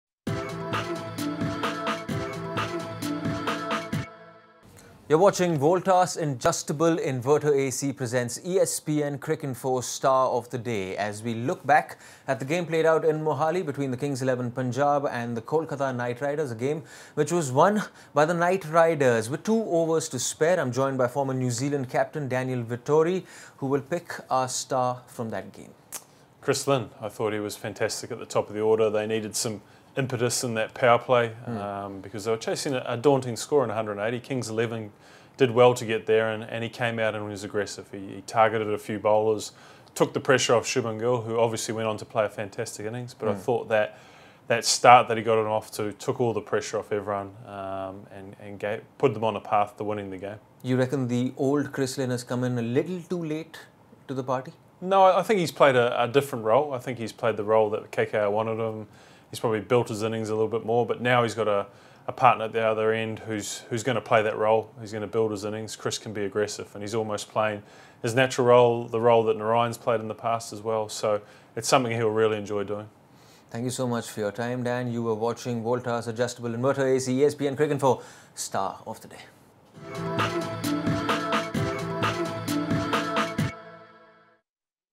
Daniel Vettori: Chris Lynn's start took the pressure off the rest of the batsmen
Daniel Vettori picks his star of the day - Chris Lynn as Kolkata beat Punjab and stay in the playoffs race